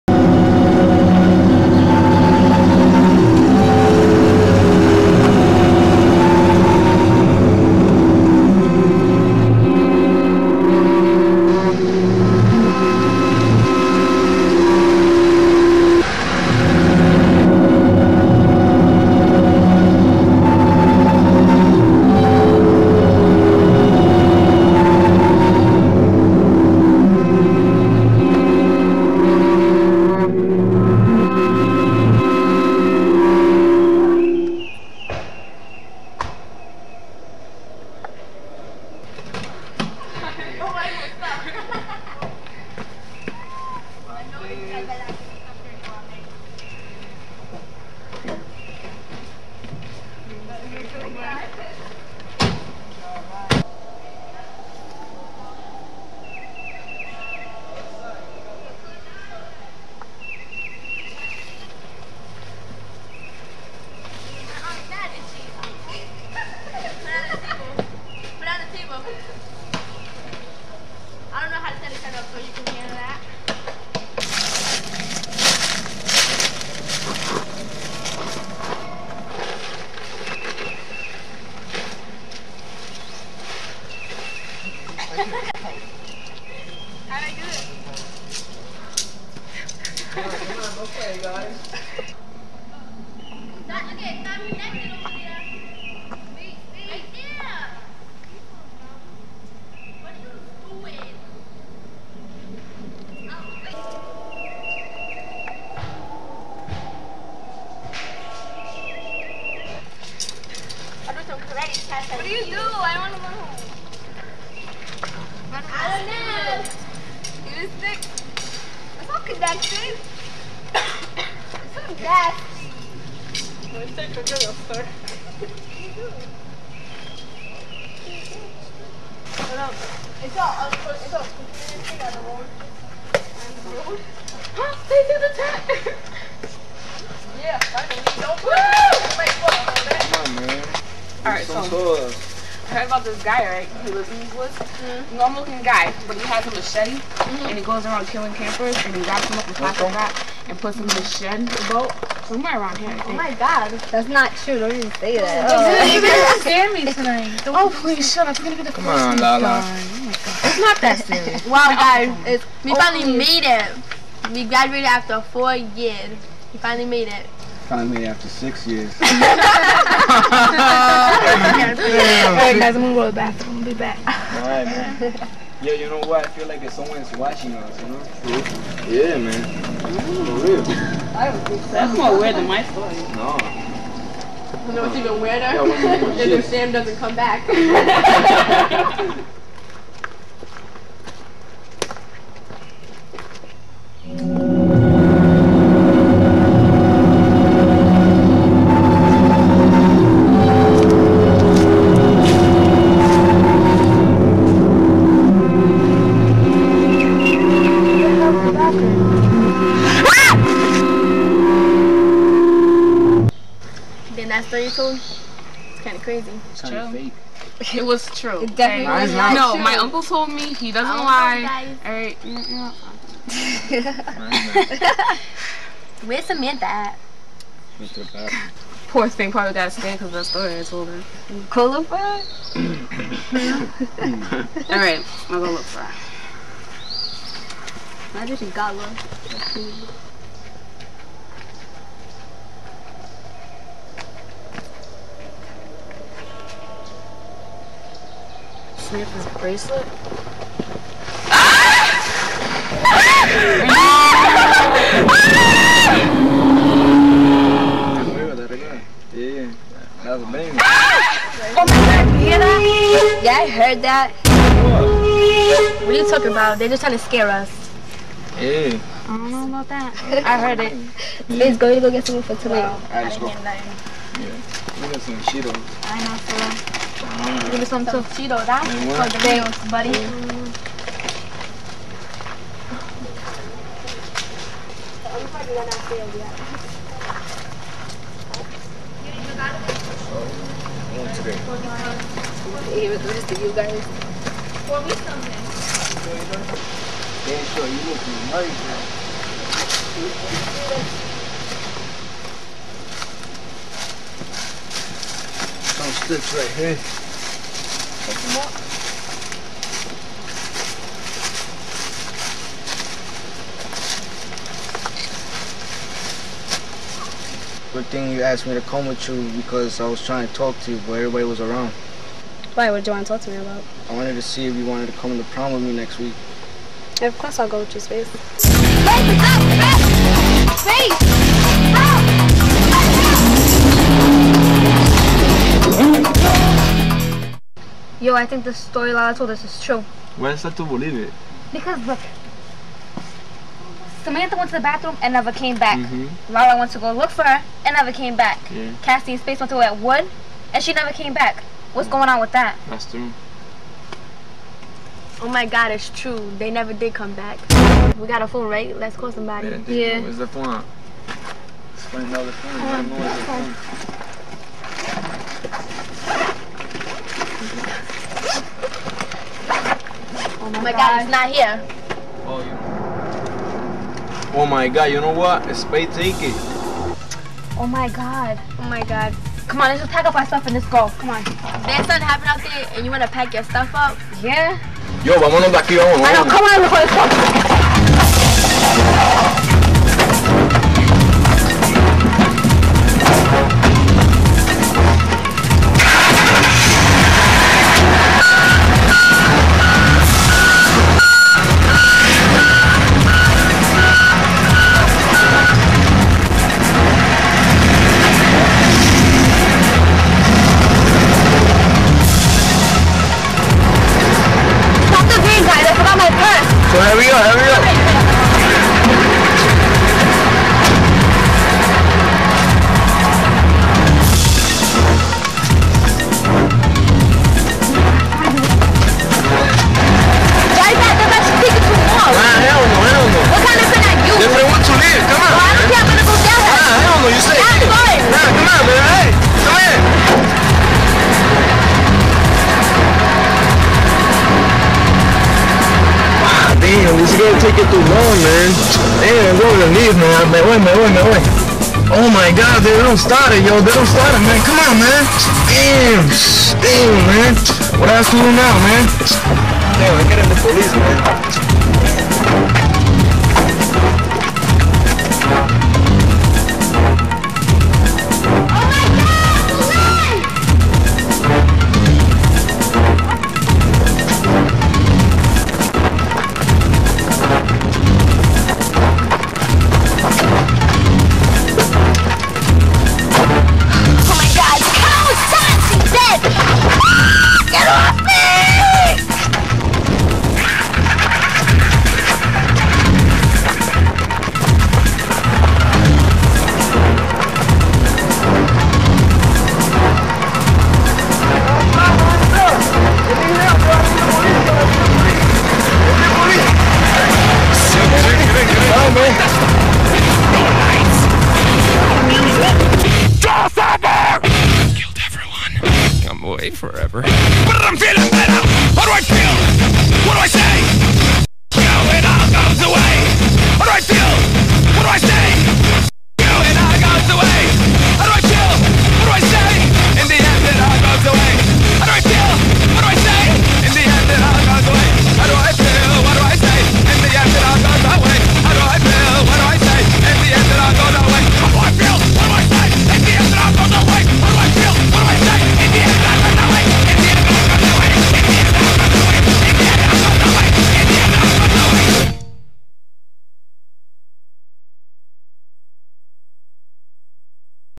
The Campin' Trip
When a group of teens go for a camping trip, they find out it's more trouble than just putting up a tent, but keeping themselves alive.